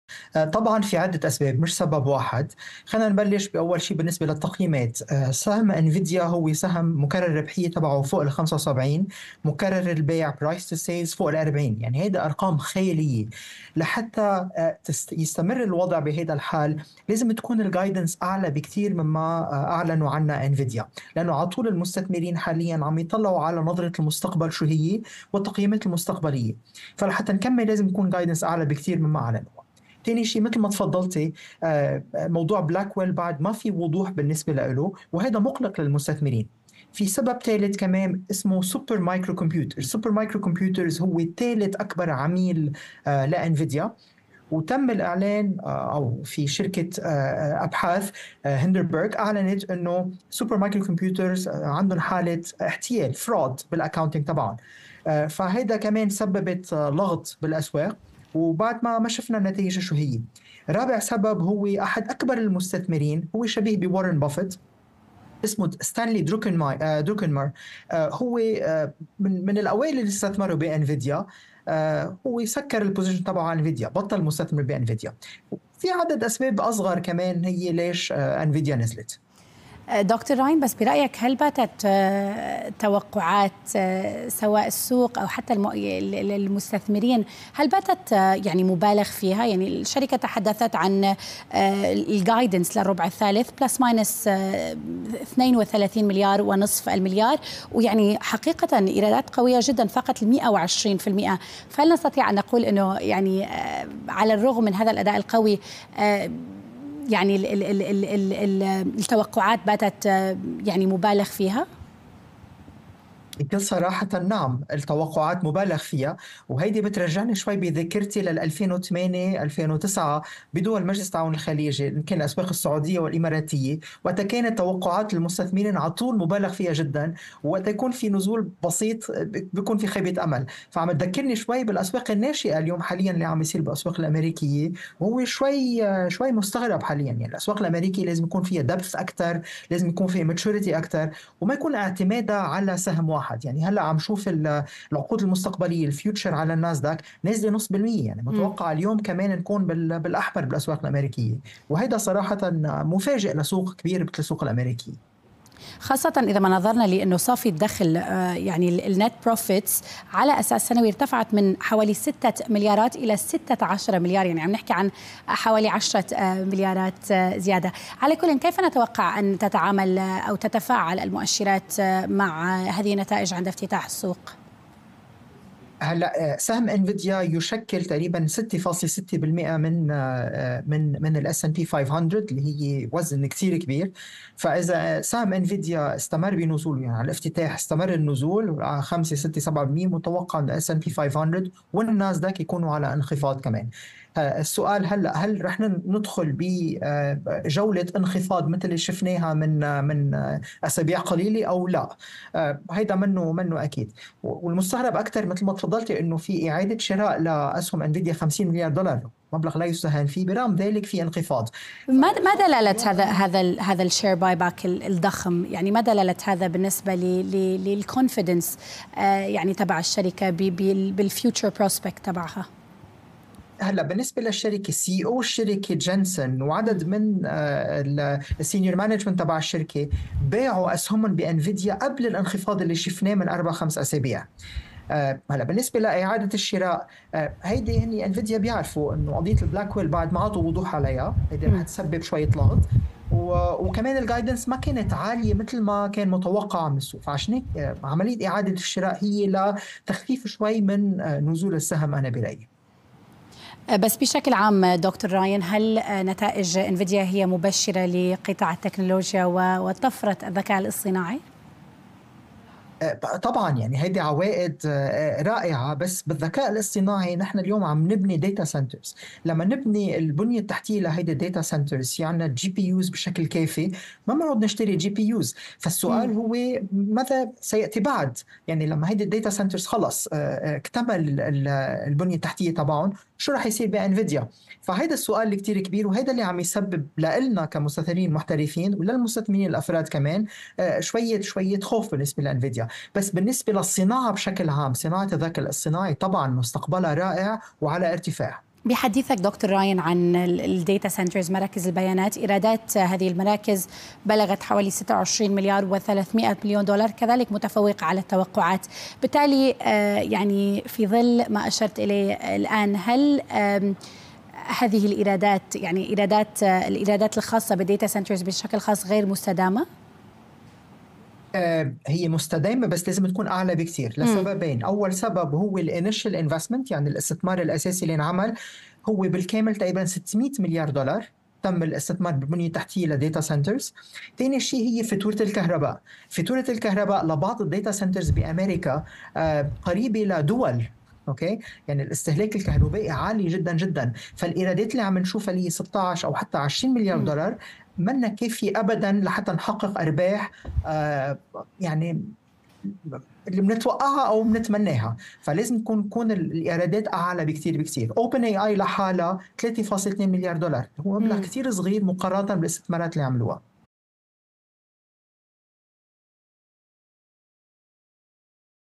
Interview on @AlArabiyaBusiness on August 29th, 2024
- There are multiple reasons why Nvidia did not rally following the earnings announcement last night, despite the beat and positive guidance:

- The guidance provided did not meet investors' expectations. When a stock is trading at more than 70x PE and more than 40x price-to-sales, investors expect future revenue to be astronomically high to justify such valuations.

- There was no real guidance on the Blackwell chip, which is instrumental for Nvidia's future.

- Nvidia's third-largest client, Micro Super Computers, has been accused of accounting fraud.

- Stanley Druckenmiller, one of the first adopters of Nvidia and among the first large investors in the company, has reduced his position in the company.

- Nvidia's CEO and other insiders have massively sold their stocks at the top before the correction that took place a few weeks ago.

- Income generated from AI is still disappointingly low, while the AI industry would need to generate more than $600 billion in revenue to justify the investments going into data centers.

- Data centers are very expensive to run from an energy perspective, with some U.S. data centers consuming more electricity than entire countries.

Yes, AI has a bright future, just like all new technologies and industries, but current insane valuations are making investors very jittery, especially since most of them are retail investors rather than institutional.